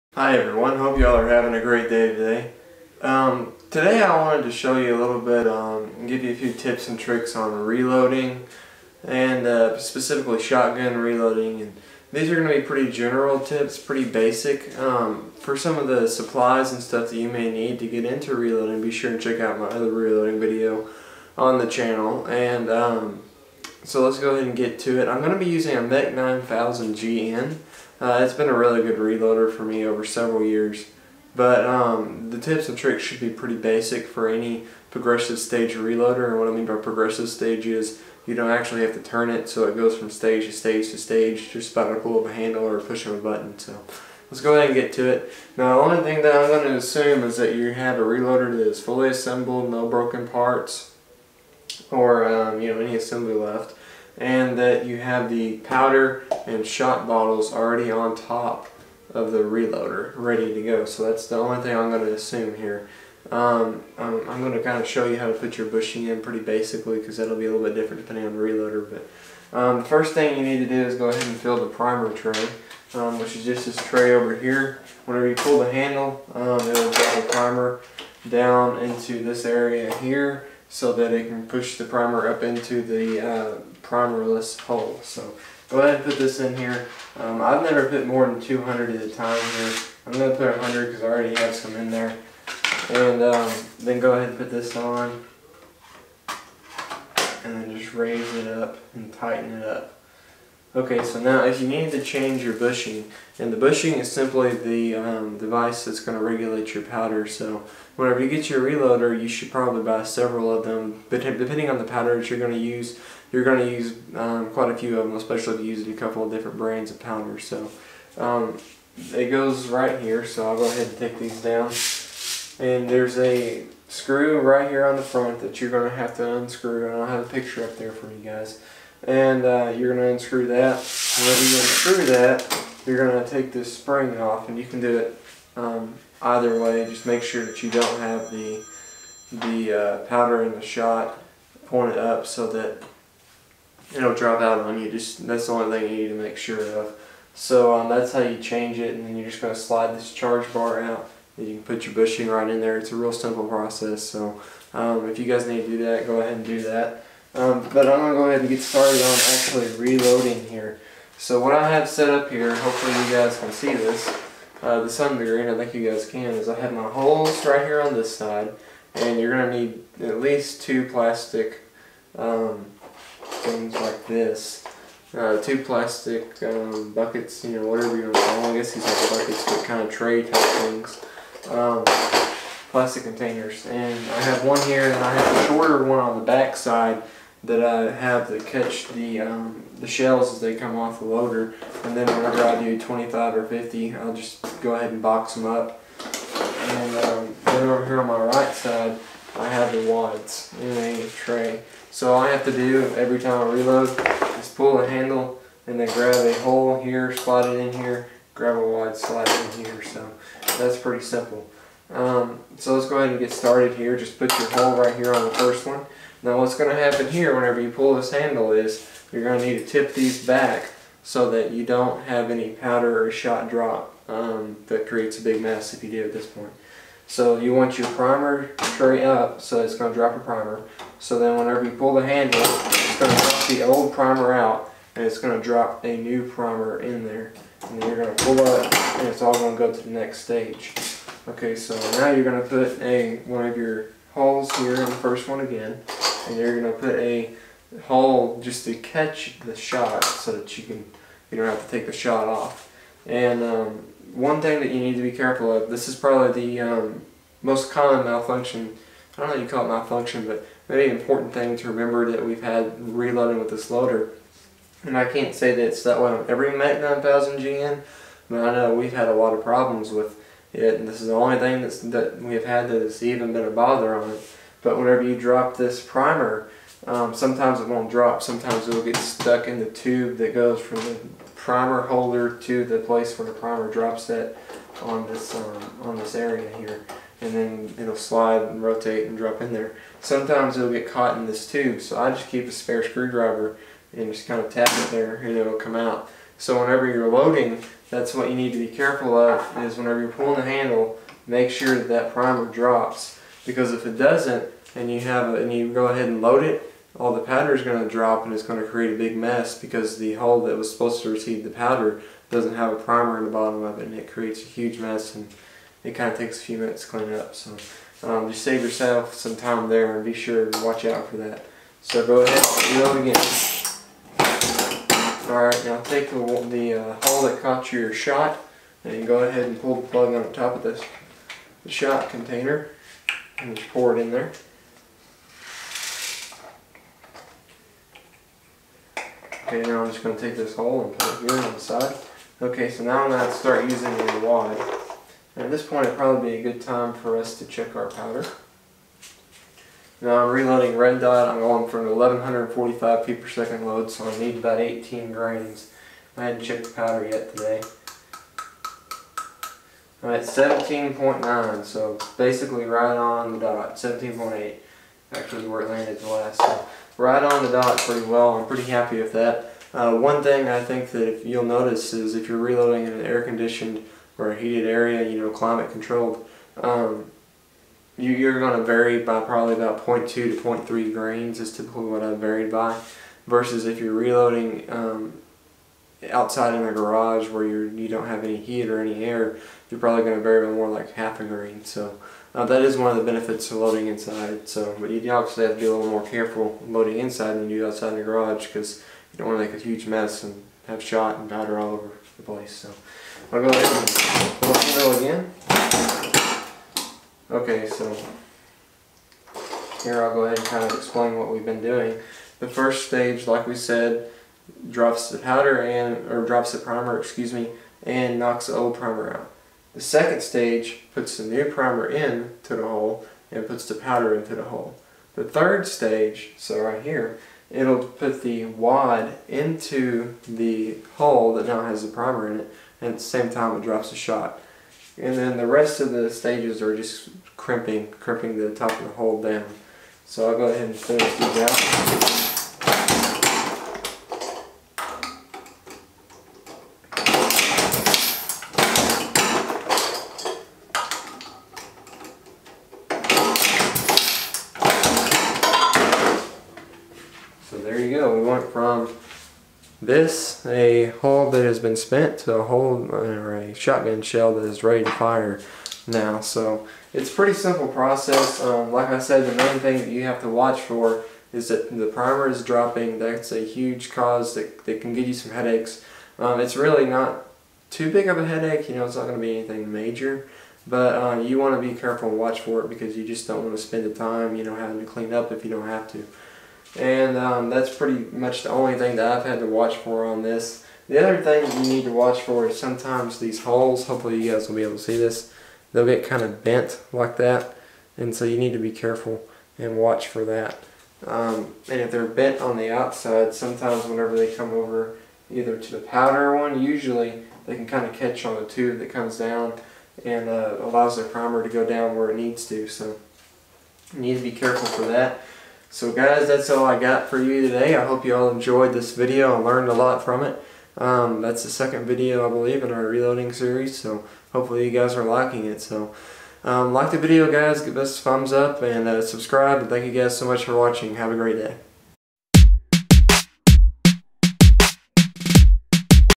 How to Reload!!!
In this video I use my MEC 9000gn to show you some of the basics of shotgun reloading.